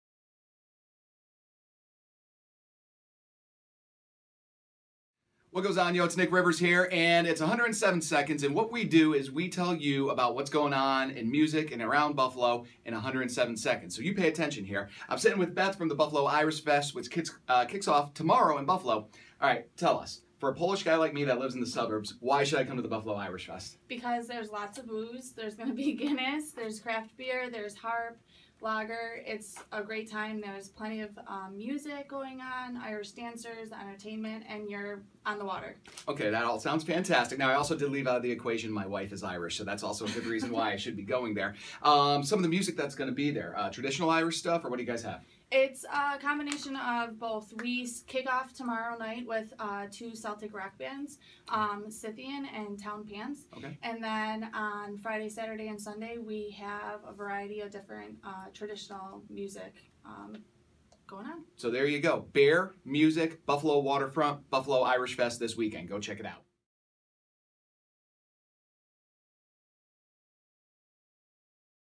107Seconds with the Buffalo Irish Fest 2017!
Nik sat down with Beth from the Buffalo Irish Festival to talk all things Buffalo Irish Festival!